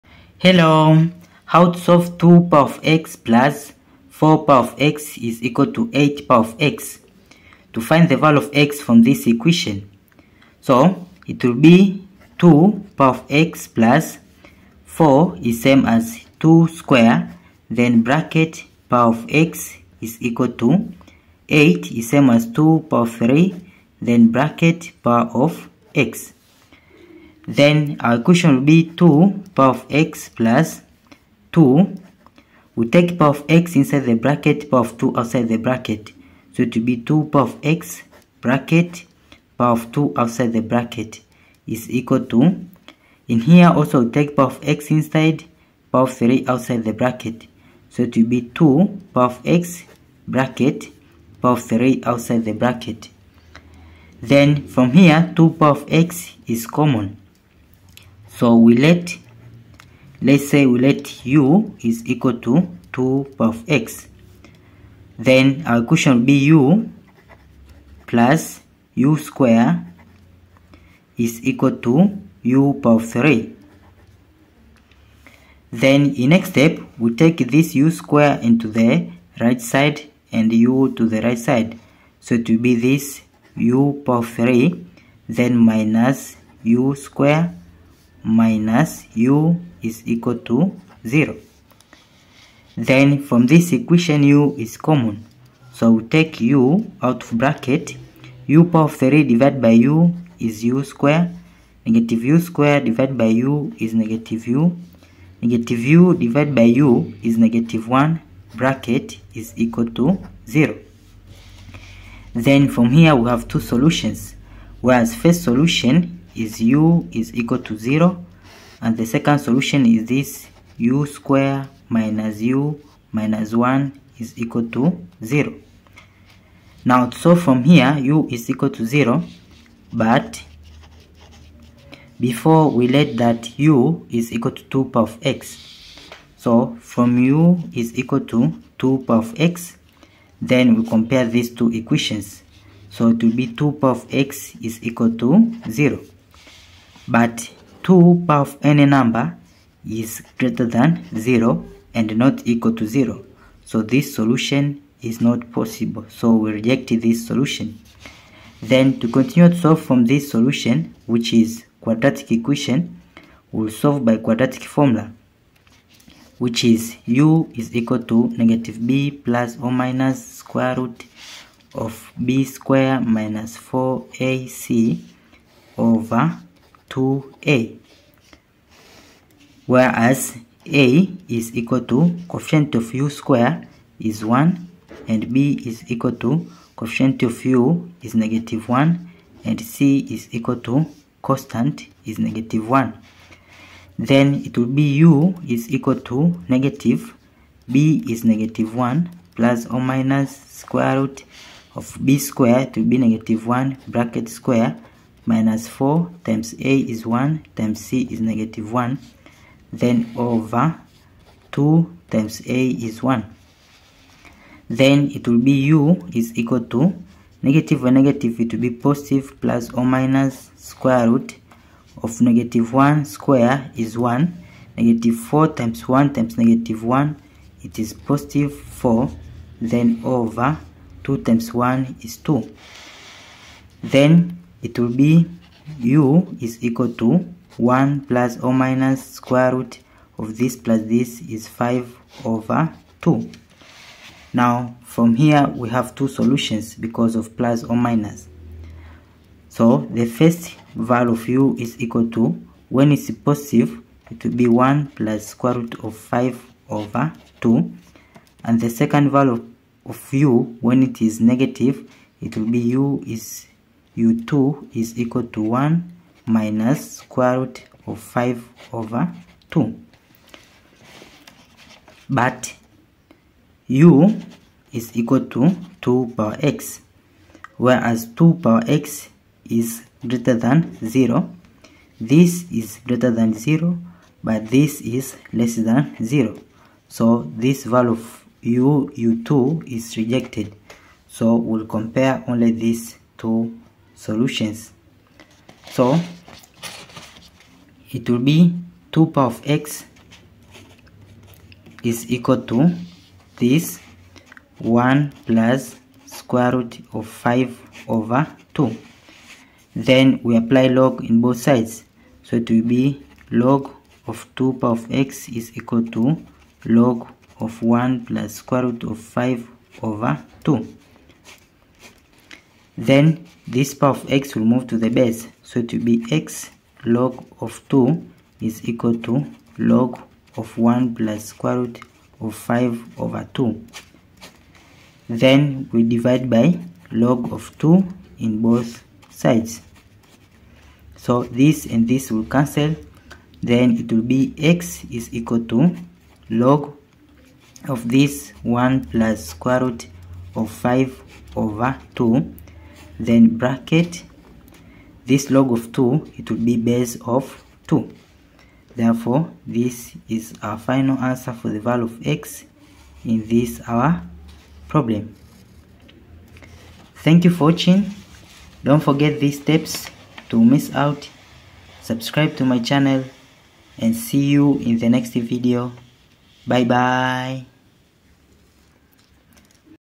Mexico - A Nice Math Olympiad Problem.
How solve 2^x + 4^x = 8^x find the value of x in this math Olympiad Problem